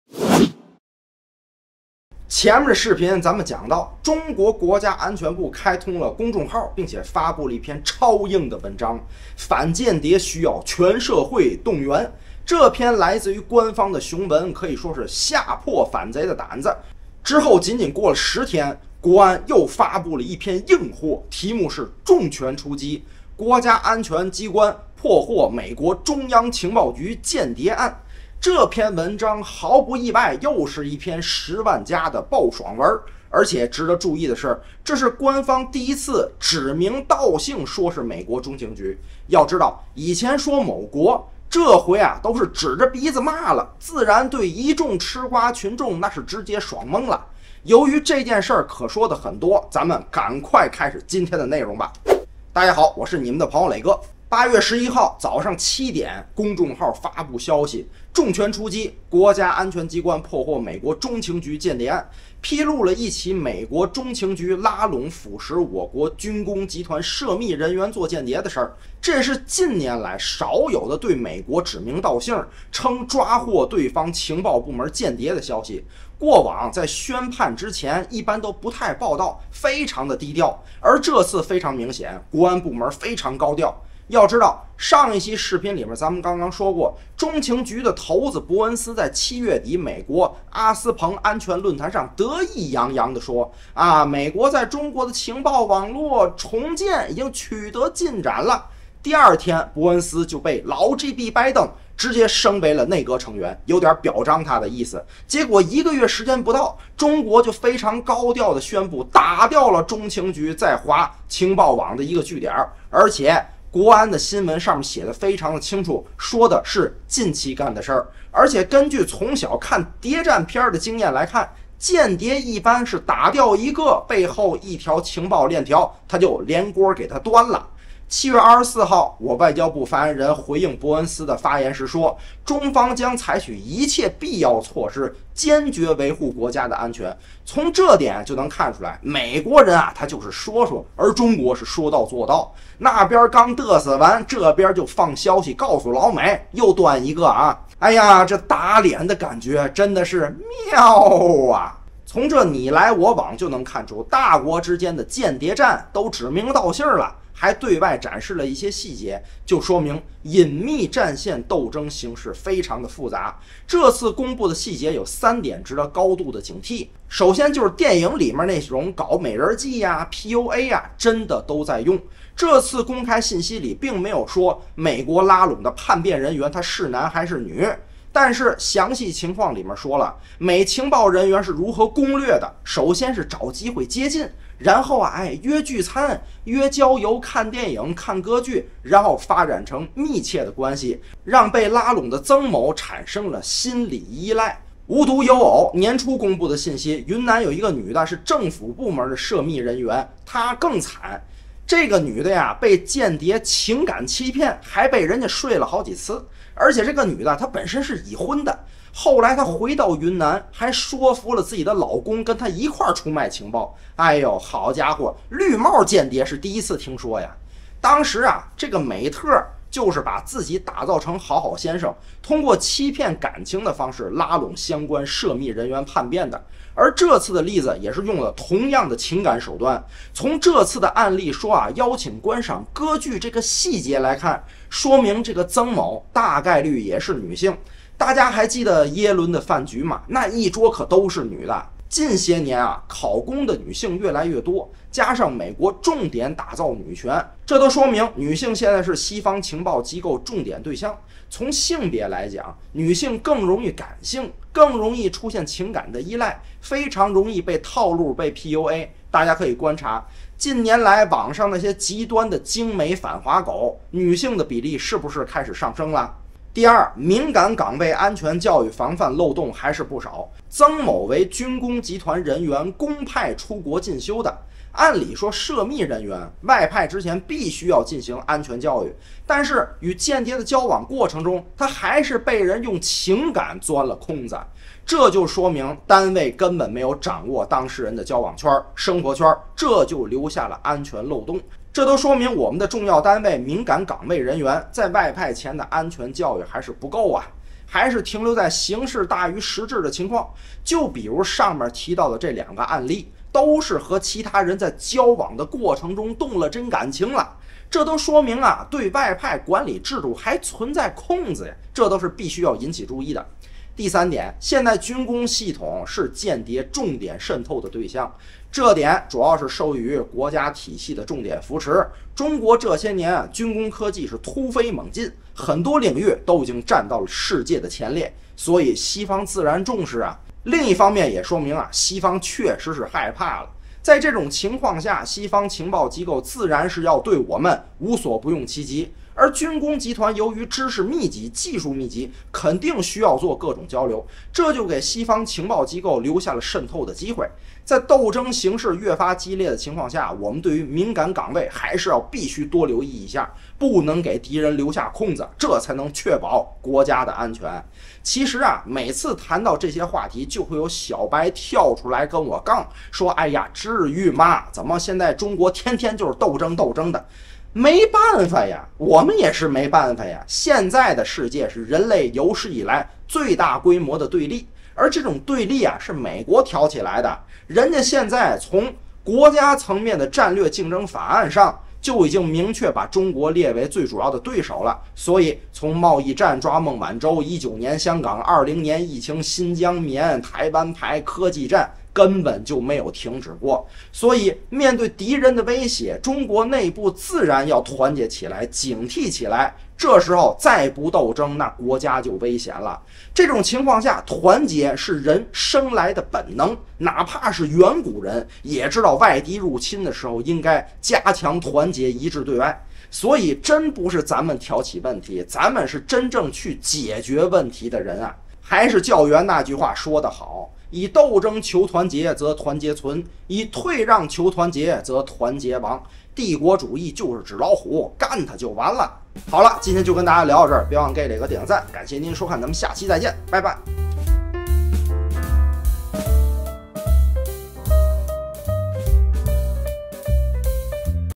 中美隐秘战线斗争加剧！反间谍战公开化！美国刚宣布重建对华情报网，中国国安就宣布抓获美中情局间谍，中国开始大谈斗争背后的原因是什么？
B站频道：磊哥聊政经
微信公众号：正直的磊哥
感谢大家的支持~~~爱你们